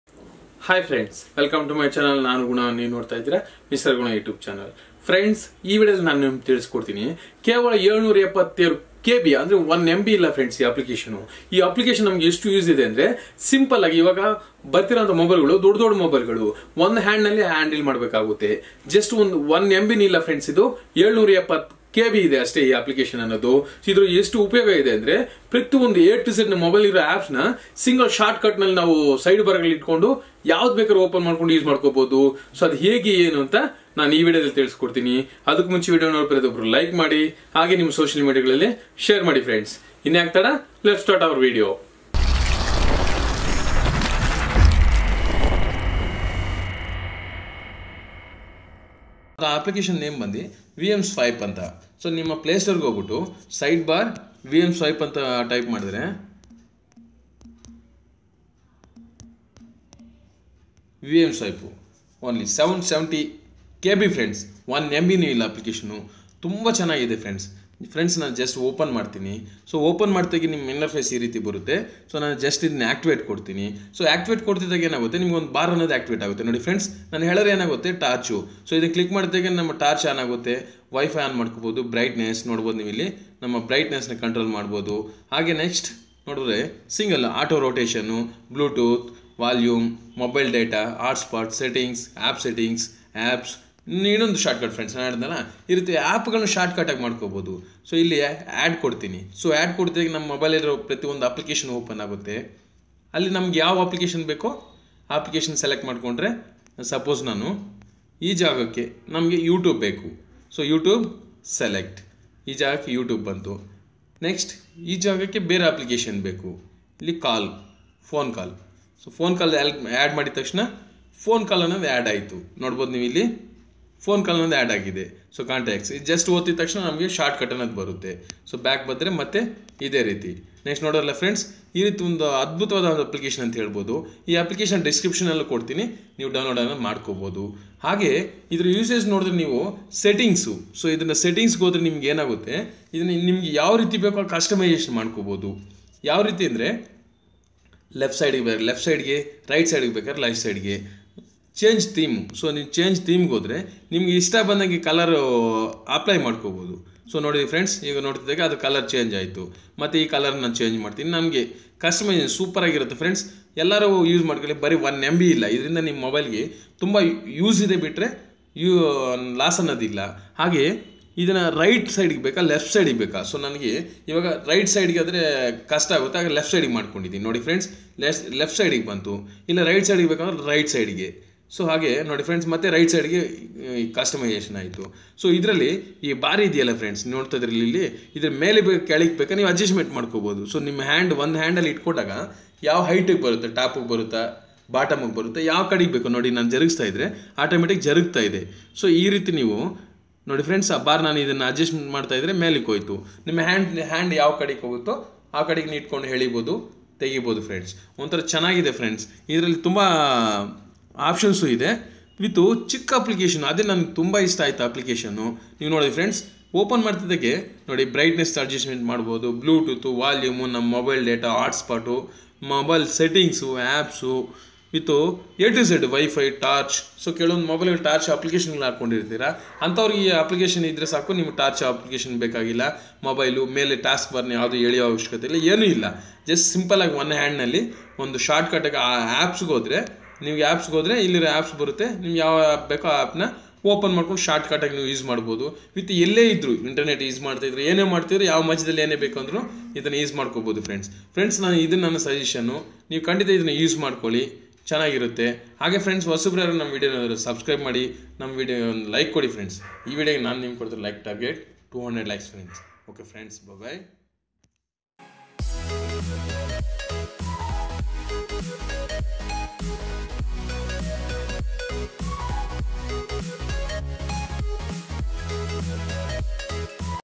Best Sidebar App For Android VmSwipe || Mister Guna-YouTube
this video i show u how to
Best Sidebar App For Android VmSwipe
IF you like this Video Please share to your Friends On Facebook or Whats app ➽♥